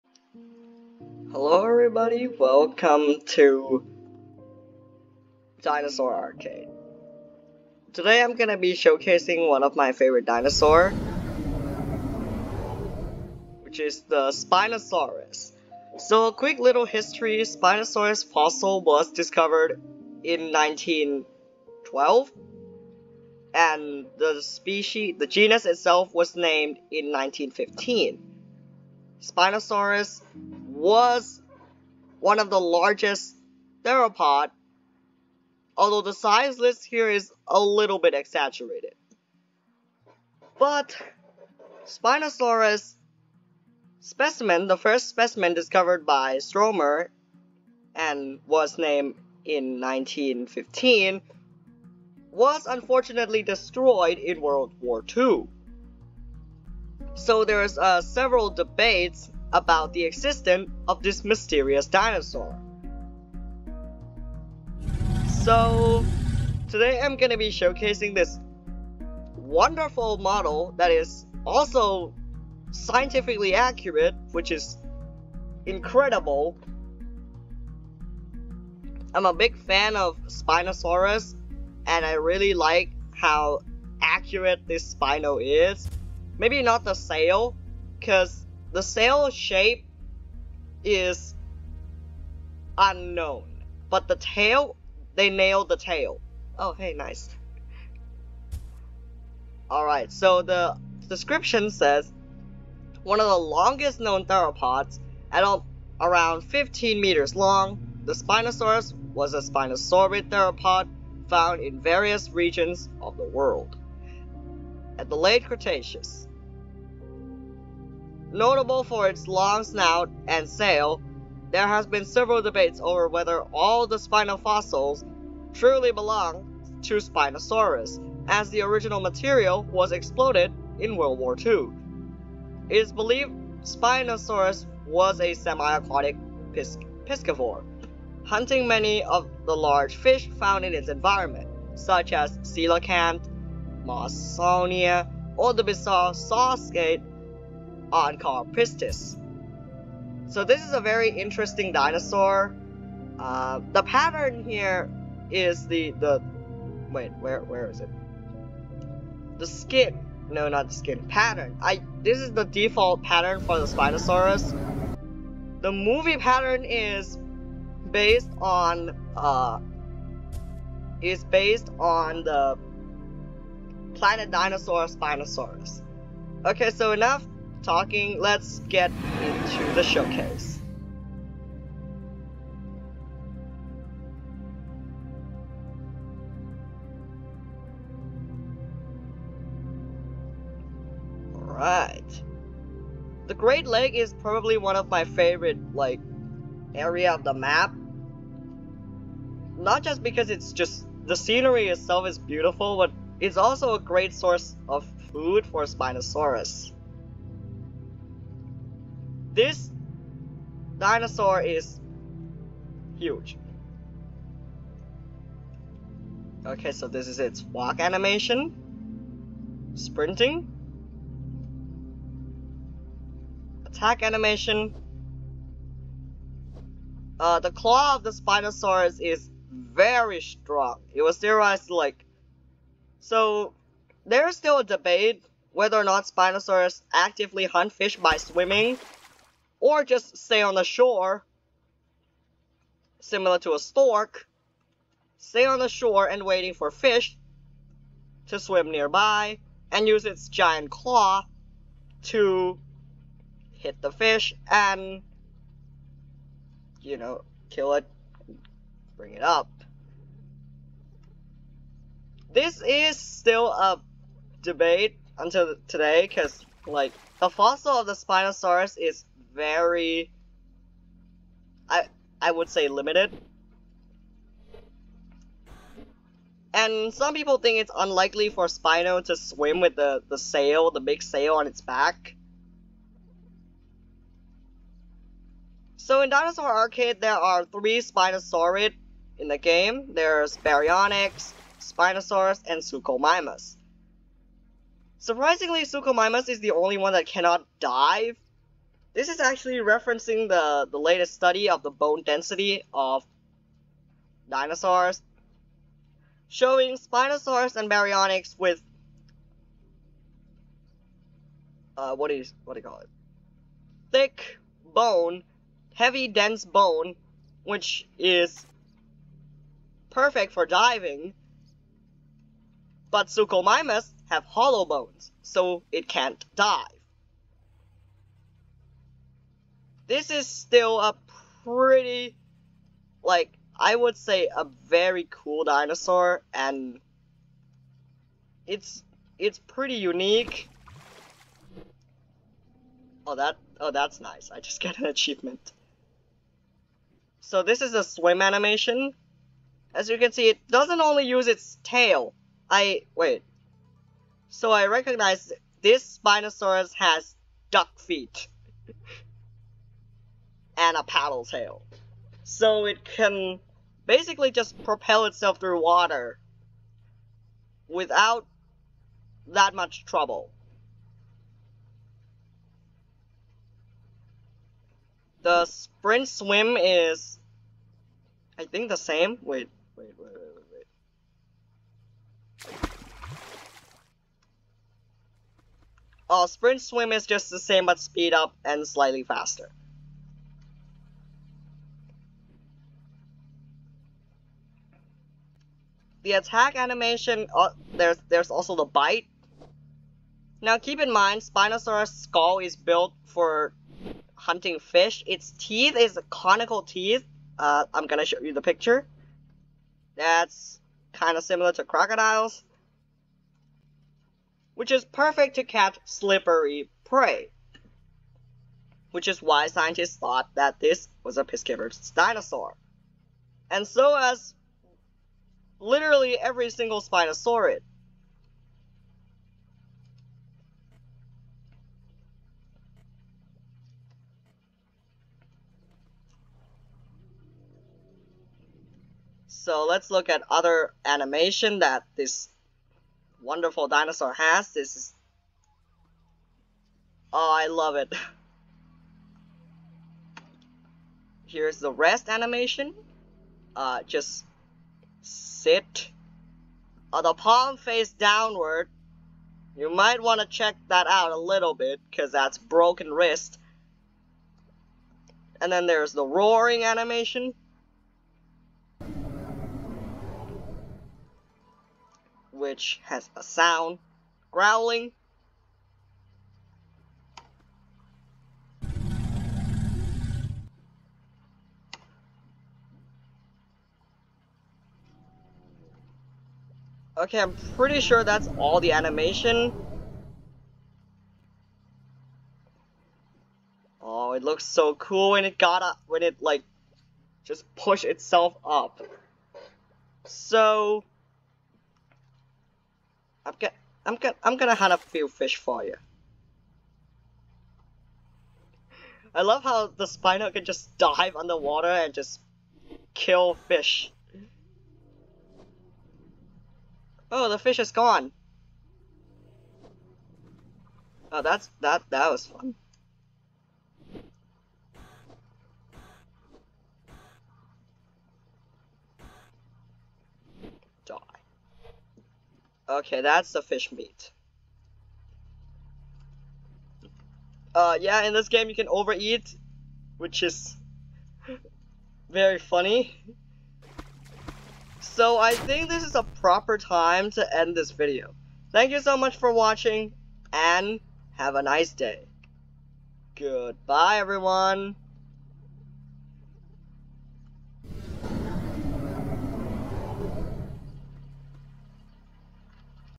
Dinosaur Arcade #1 Spinosaurus, Ruler of River
One of the longest known theropods, at around 15 meters long, Spinosaurus was a Spinosaurid theropods found in various regions of the world, at the Late Cretaceous. Notable for its long snout and sail, there have been several debates over whether all the Spinosaurus fossils truly belong to Spinosaurus, as the original material was exploded in WW2. It's believed Spinosaurus was a semi-aquatic piscivore, hunting many of the large fish found in its environments, such as the coelacanth Mawsonia or the bizzare "Saw-Skate" Onchopristis.